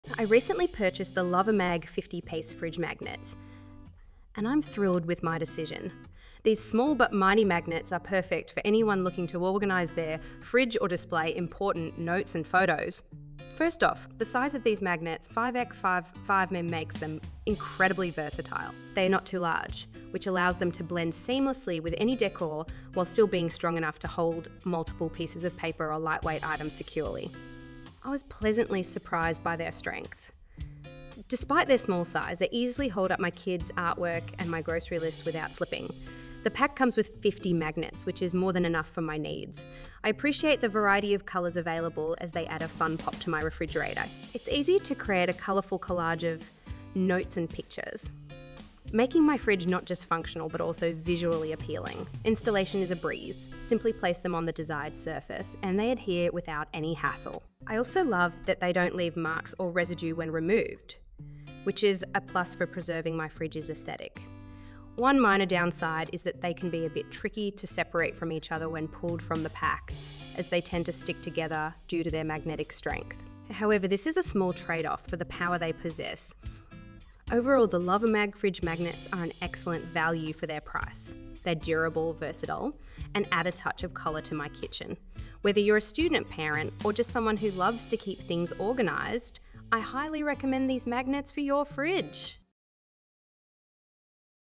LOVIMAG 50Pcs Fridge Magnets 5x5x5mm Refrigerator Magnets Magnets for Fridge Magnets Review
I recently purchased the LOVIMAG 50Pcs Fridge Magnets, and I'm thrilled with my decision. These small but mighty magnets are perfect for anyone looking to organize their fridge or display important notes and photos.

First off, the size of these magnets—5x5x5mm—makes them incredibly versatile. They are not too large, which allows them to blend seamlessly with any décor while still being strong enough to hold multiple pieces of paper or lightweight items securely. I was pleasantly surprised by their strength; despite their small size, they easily hold up my kids’ artwork and my grocery lists without slipping.

The pack comes with 50 magnets, which is more than enough for my needs. I appreciate the variety of colors available, as they add a fun pop to my refrigerator. It's easy to create a colorful collage of notes and pictures, making my fridge not just functional but also visually appealing.

Installation is a breeze; simply place them on the desired surface, and they adhere without any hassle. I also love that they don’t leave marks or residue when removed, which is a plus for preserving my fridge's aesthetic.

One minor downside is that they can be a bit tricky to separate from each other when pulled from the pack, as they tend to stick together due to their magnetic strength. However, this is a small trade-off for the power they possess.

Overall, the LOVIMAG fridge magnets are an excellent value for their price. They’re durable, versatile, and add a touch of color to my kitchen. Whether you're a student, parent, or just someone who loves to keep things organized, I highly recommend these magnets for your fridge!